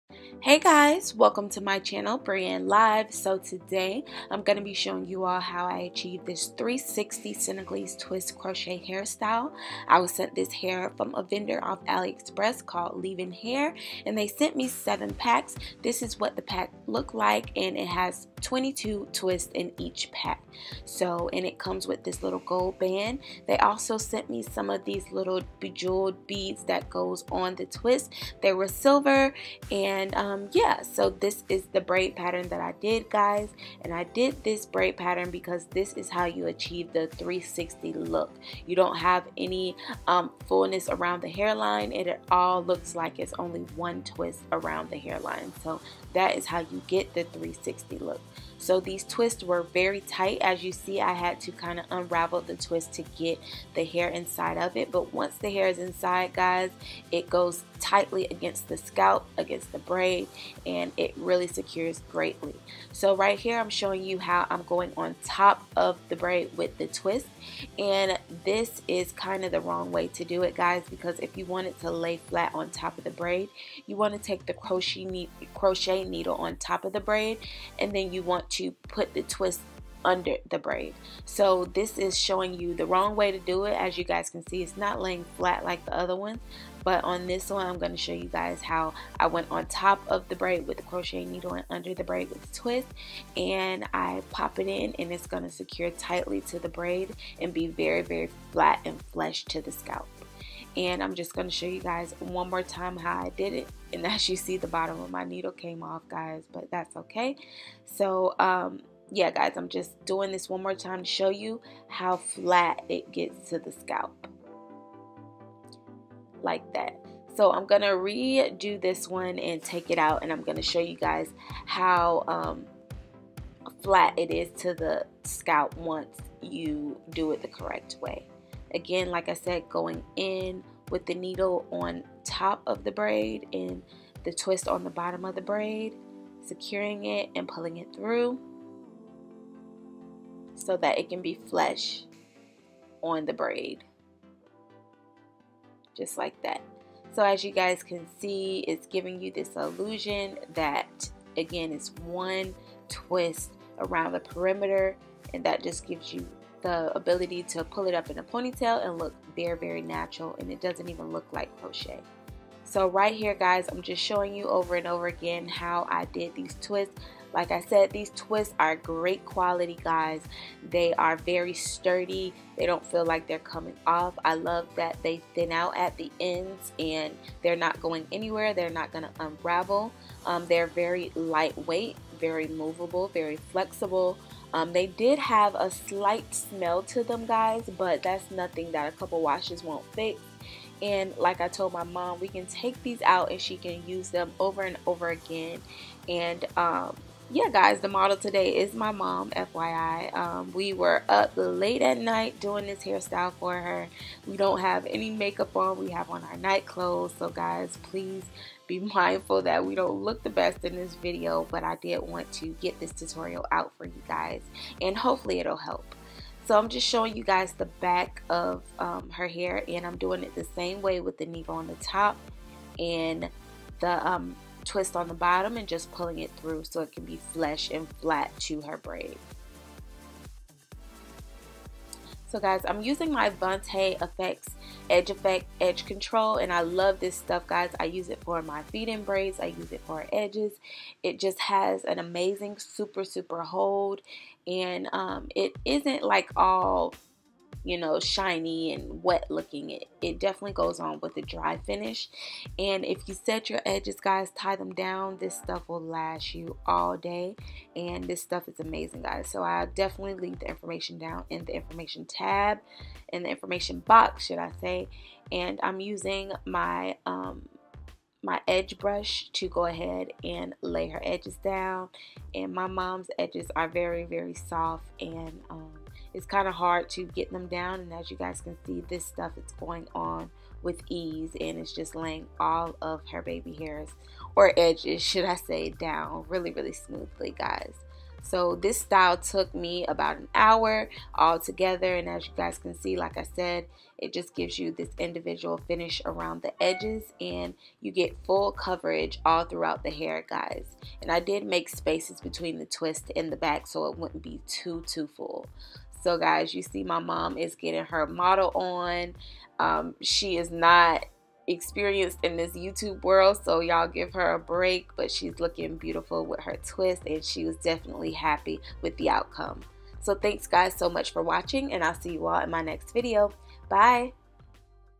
Senegalese Twist Crochet In An Hour + Braid Pattern Ft. MAMA!
⃟Be sure to watch in HD. Juicy Details Below!

⃟Hey Loves! So I did my moms hair and I wanted to show you all the method i use to make my crochet styles look at natural as possible! You can check out more of my styles on my hair instagram (@Wigfallstyles) I have a burn on my arm so if you see something on my arm that it was it is! My mom and I were relaxing and chilling in this video! I hope you all enjoy this video leave any questions you have down below.

⃟Hair Used:
Leeven Hair
7 packs color #4 12inch
Senegalese twist
Shipping did take a while
3.29/ bag

 ⃟Products In This Video:
Vonte Edge Effect
Last Touch Edge Brush
Coconut Oil

 ⃟Tech Stuff:
-Camera: Canon
-Editing: iMovie
-Lighting: Limo Studio Lighting